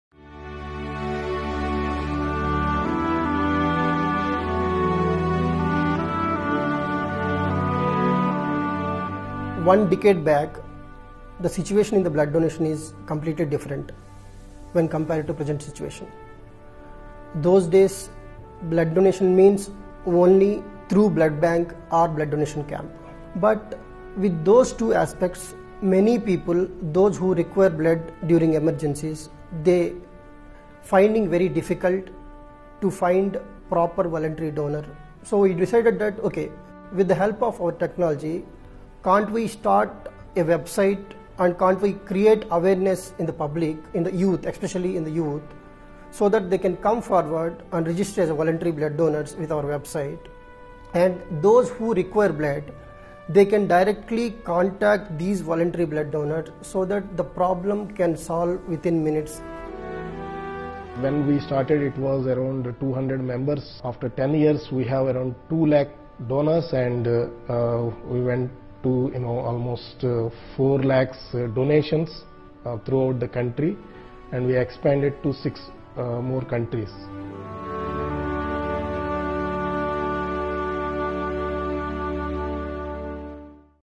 DigitalTrailblazer Award 2016 to Friends2Support.org
Friends2support.org is world's largest online voluntary blood donors network which has operations in 6 countries including Malaysia, Sri Lanks, Bangladesh, Nepal,  Yemen and India